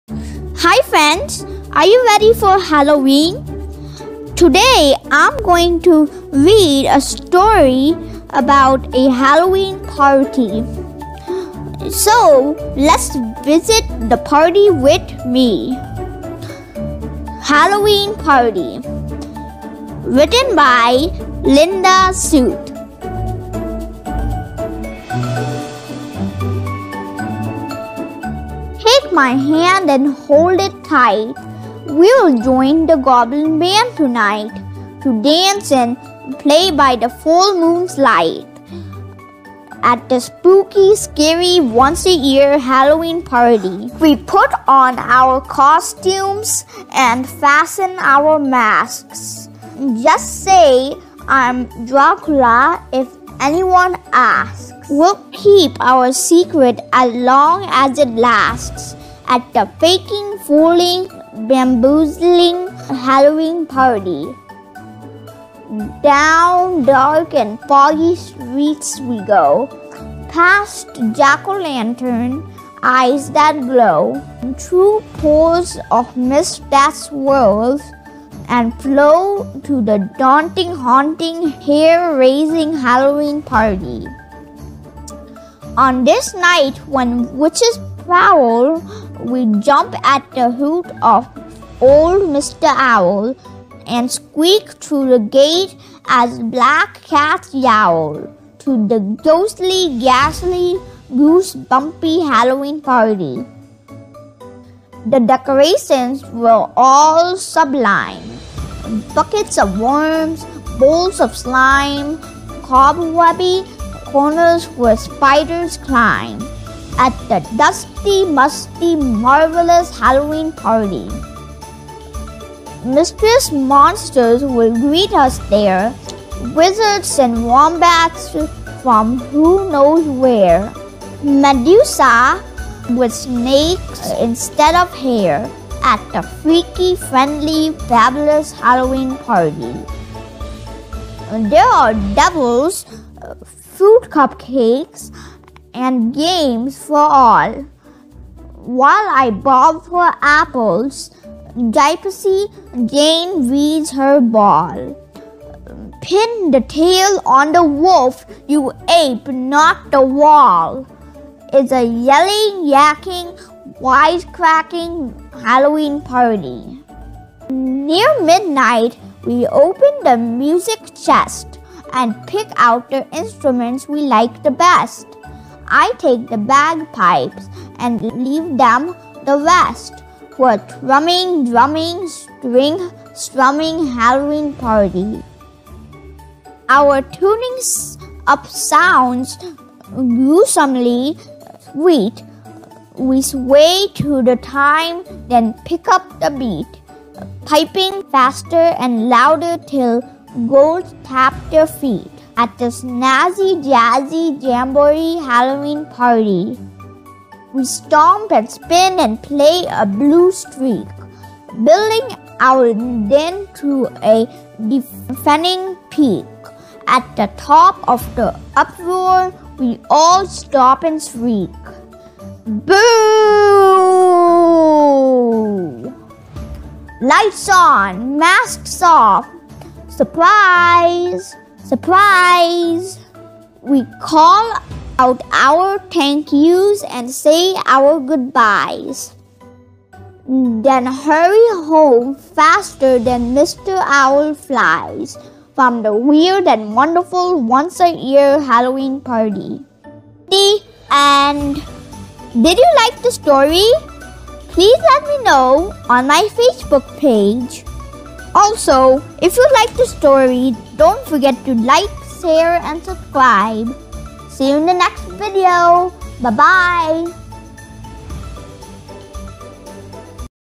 Halloween Party Read Aloud || Halloween Stories For Kids || Halloween story Read Aloud
Halloween Part by Linda Shute will bring you to a daunting, haunting, hair raising Halloween Party.
So who is here to experience the Halloween Party? Lets read aloud.
Halloween Party is a wonderful book for kids to read aloud in Halloween.